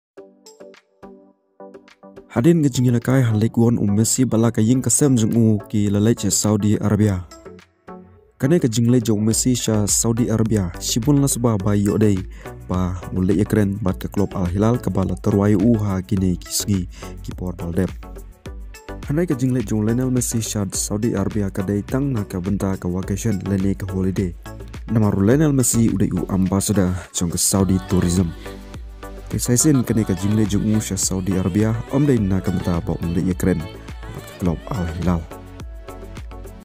Namar ba u Messi u la don mynta sha Saudi Arabia shibun la Suba ioh jia ba u leit iakren bad ka Al..
Namar ba u Messi u la don mynta sha Saudi Arabia shibun la Suba ioh jia ba u leit iakren bad ka Club Al Hilal kaba la Tyrwa ia u, hynrei??